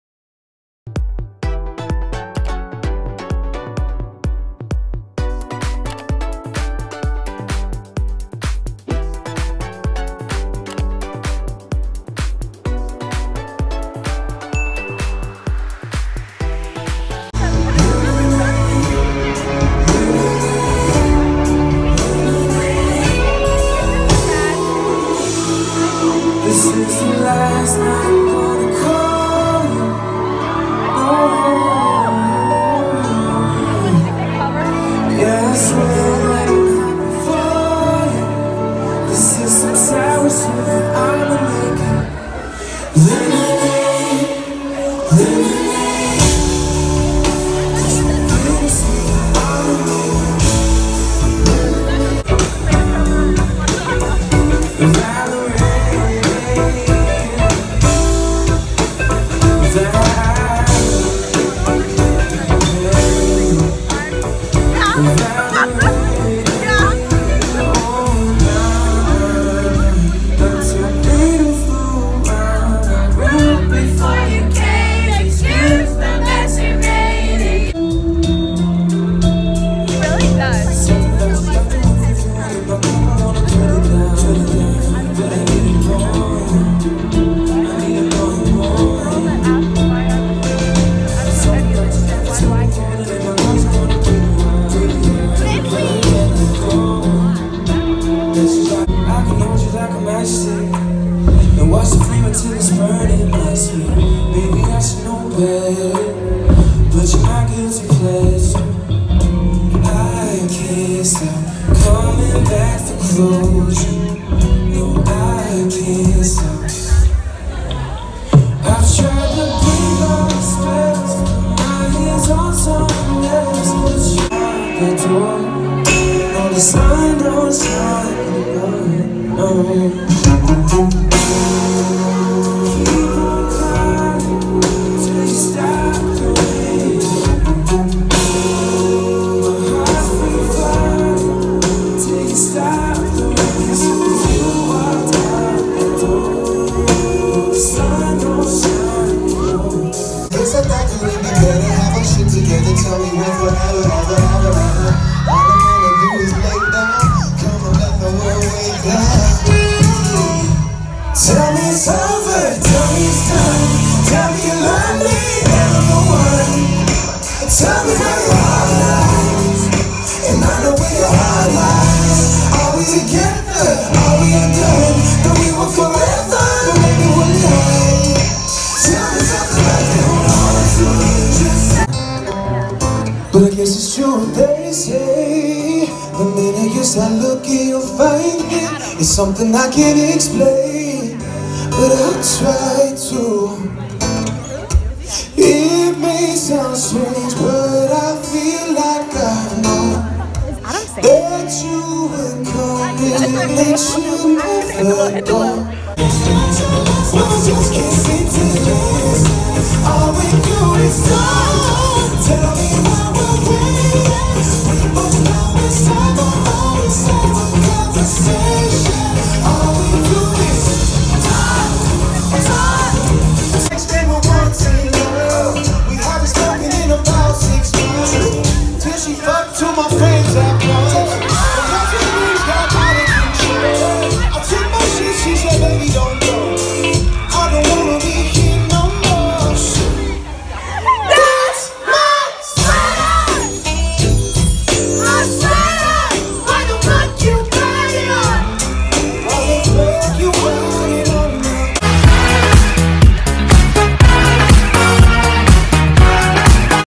Alec Bailey & Spencer Sutherland - April 30th, 2019 - Cleveland, OH
They performed at Jack and Jack’s show! Their video will be uploaded next!